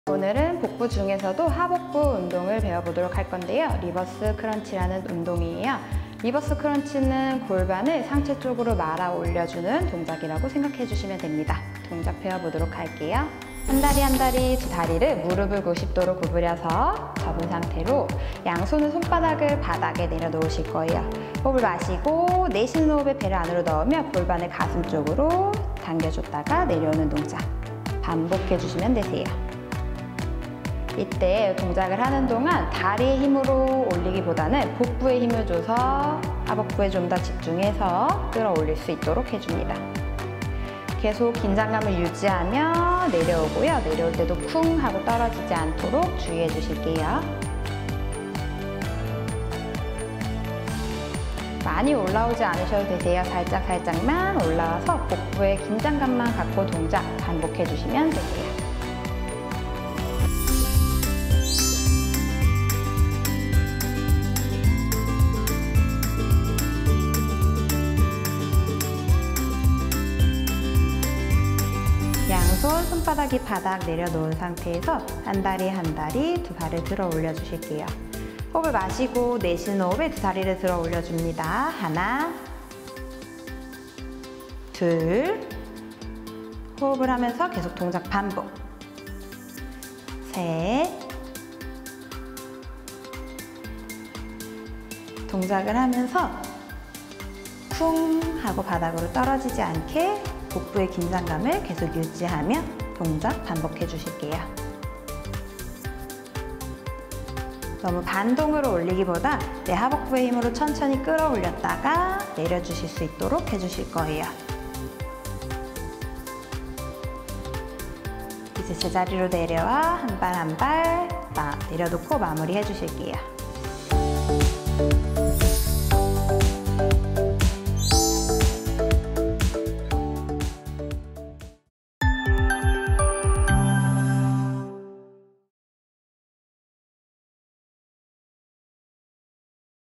리버스 크런치 / 예뻐지는 1분습관 - 1분홈트
오늘은 복부 중에서도 하복부 운동인 리버스 크런치입니다.
골반을 상체 쪽으로 말아 올리는 동작입니다.

월~금 오전 08:30 ~ 09:30